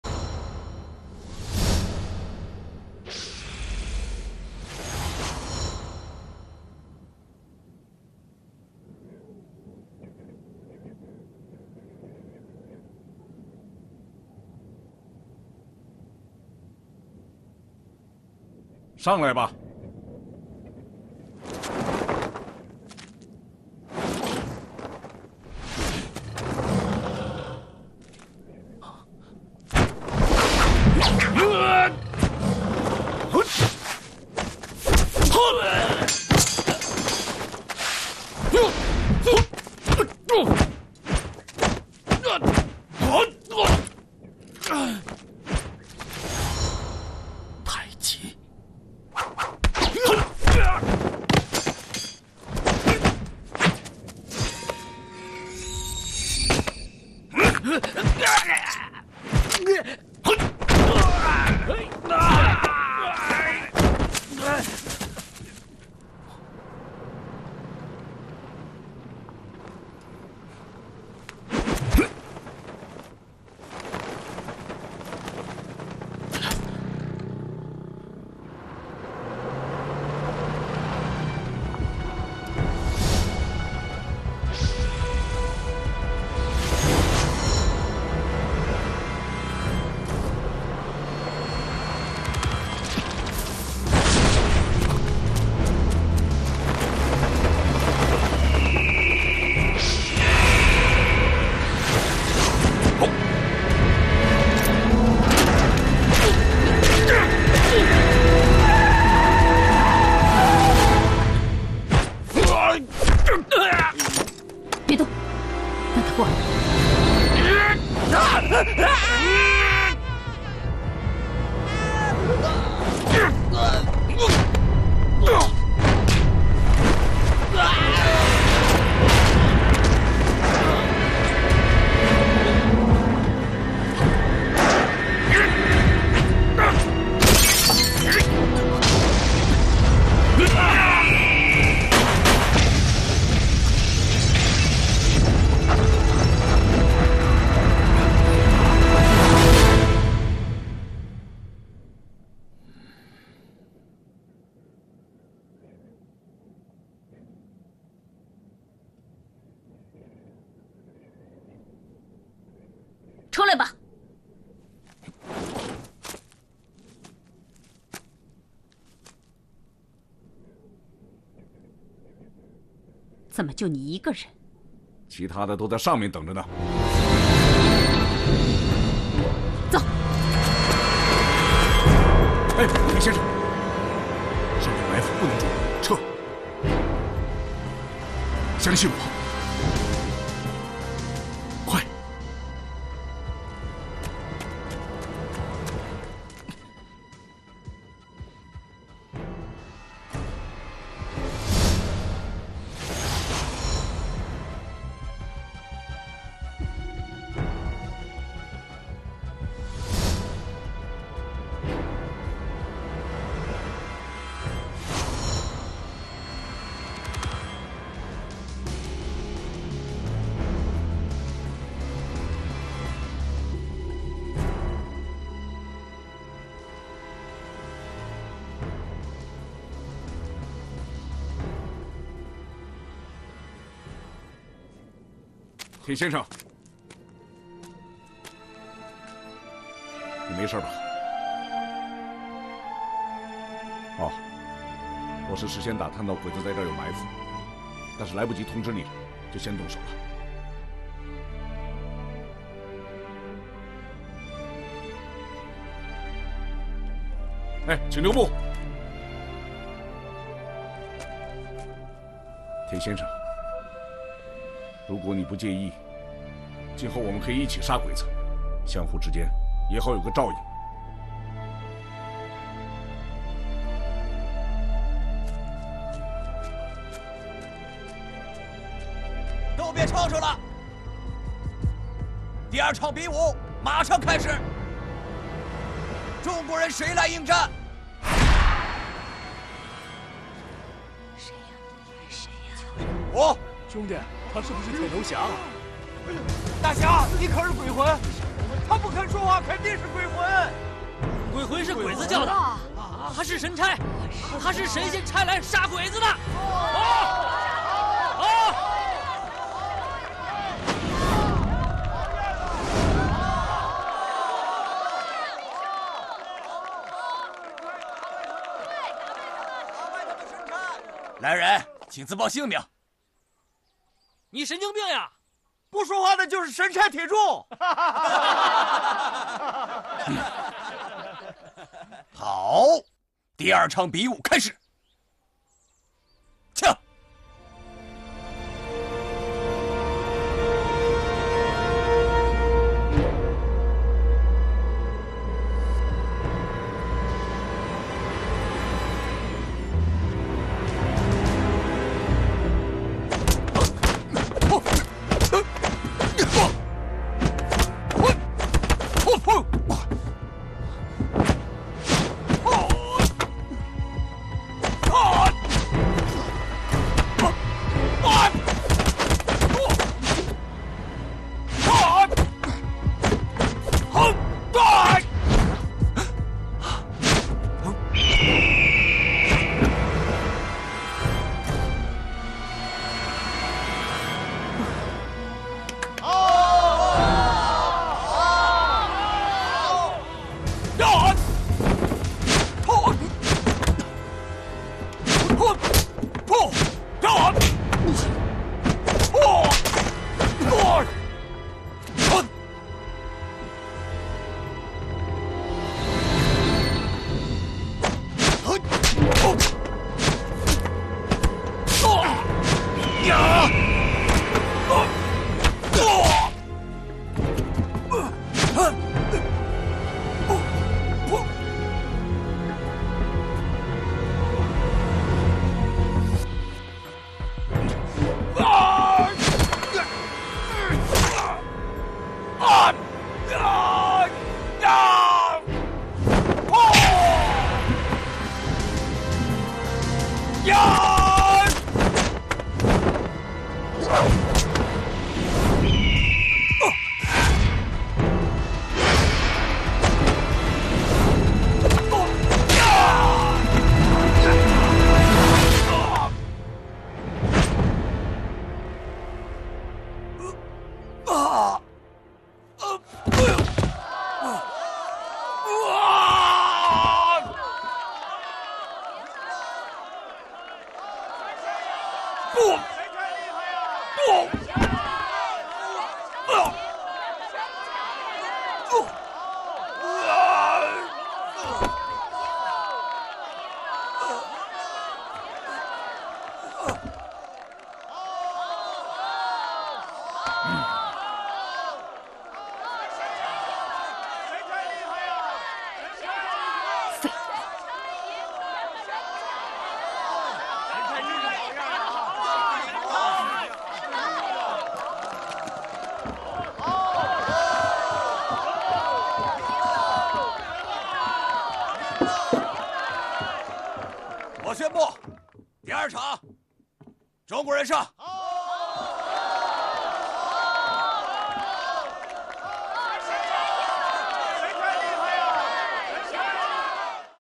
日軍設陷阱伏擊村民，不料村民是功夫高手！
krqx 1